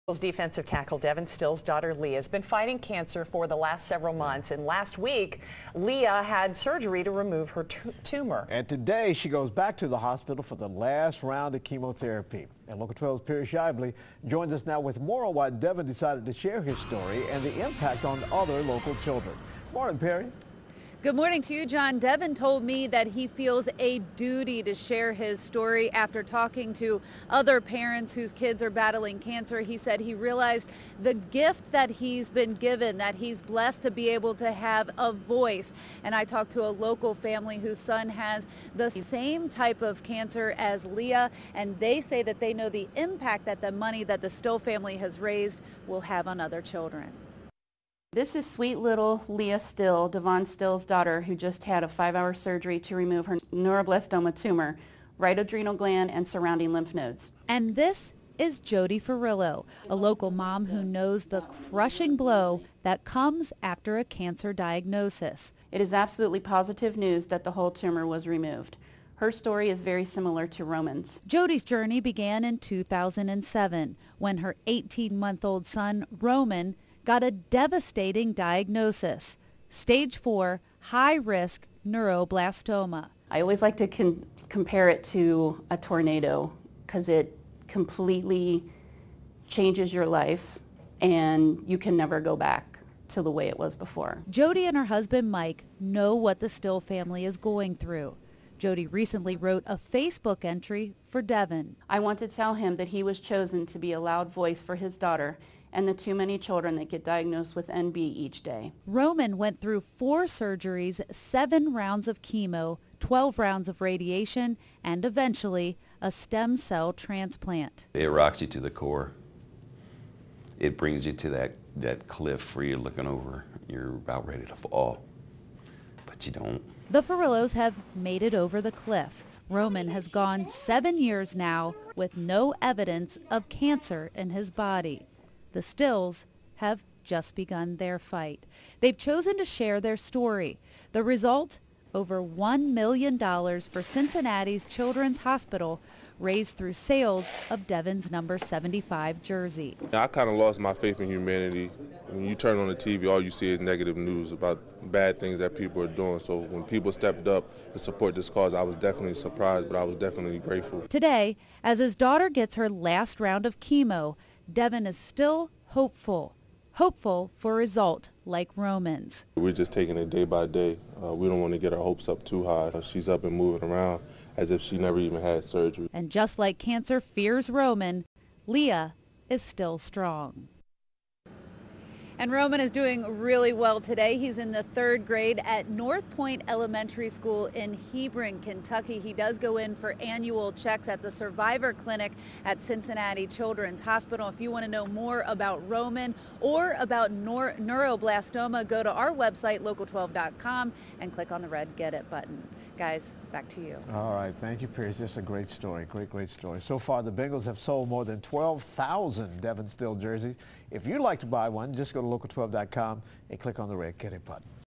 Leah Still's fight against cancer inspires oth
CINCINNATI (Perry Schaible) -- Leah Still, Devon Still's daughter, who just had a five-hour surgery to remove her Neuroblastoma tumor, right adrenal gland and surrounding lymph nodes, is inspiring others with the same type of cancer.

Jody Fariello is a local mom who knows the crushing blow that comes after a cancer diagnosis.

"It is absolutely positive news that the whole tumor was removed. Her story is very similar to Roman's," Jody said.

Jody's journey began in 2007 when her 18 month old son Roman got a devastating diagnosis, stage 4 high-risk Neuroblastoma.

"I always like to compare it to a tornado because it completely changes your life and you can never go back to the way it was before," Jody said.

Jody and her husband Mike know what the Still family is going through. Jody recently wrote a Facebook entry for Devon.

Jody said, "I want to tell him that he was chosen to be a loud voice for his daughter and the too many children that get diagnosed with NB each day."

Roman went through four surgeries, seven rounds of chemo, 12 rounds of radiation and eventually a stem cell transplant.

Mike Fariello, Roman's dad, said, "It absolutely rocks you to the core. It brings you to that cliff where you are looking over and are about ready to fall, but you don't."

The Fariellos have made it over the cliff. Roman has gone seven years with no evidence of cancer in his body.

The stills have just begun their fight. They've chosen to share their story. The result, over $1 million for Cincinnati Children's Hospital raised through sales of Devon's